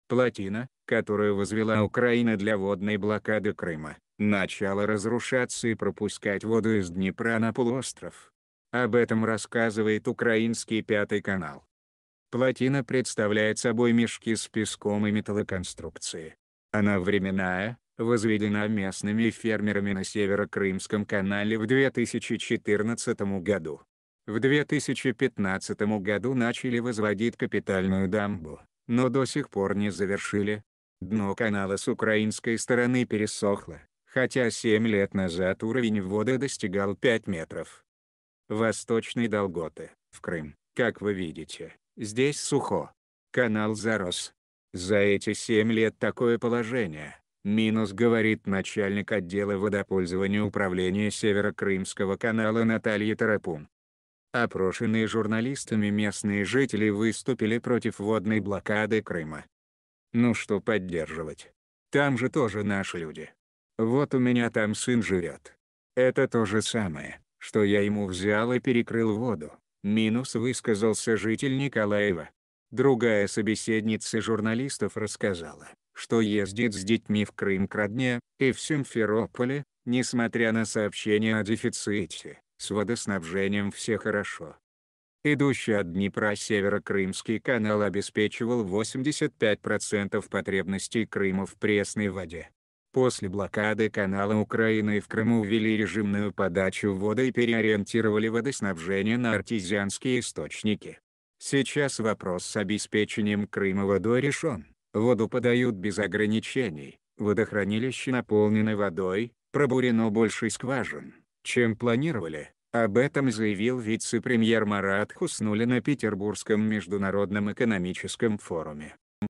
В Крым проникла "запрещенная" днепровская вода
Дамба, перекрывающая Северо-Крымский канал, начала разрушаться, и вода пошла на полуостров.